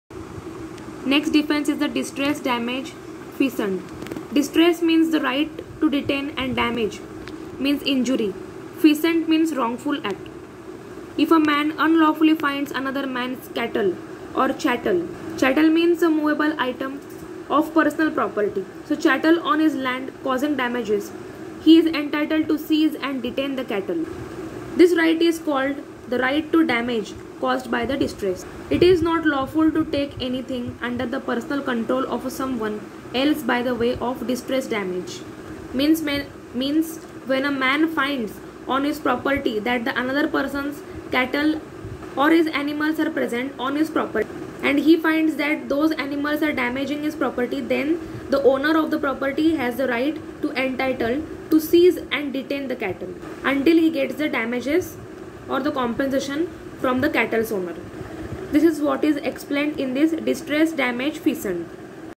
Defences: Distress Damage Feasant (Tort ) (5th Sem) (Part 6)
T.Y. BLS. LLB  (5th Semester)

Subject  =  Tort

Lesson =   Torts Relating To Immovable Property

(Part 6)